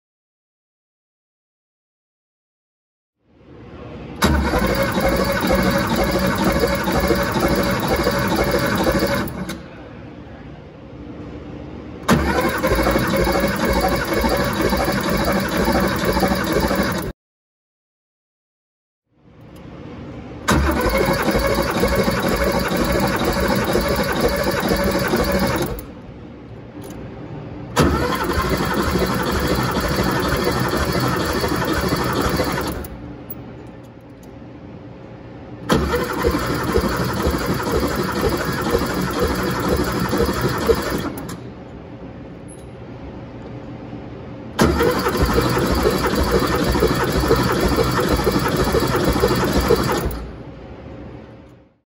1984 Porsche 911 Carrera Compression Test
Cylinder compression test on 1984 Porsche 911 Carrera for BaT auction.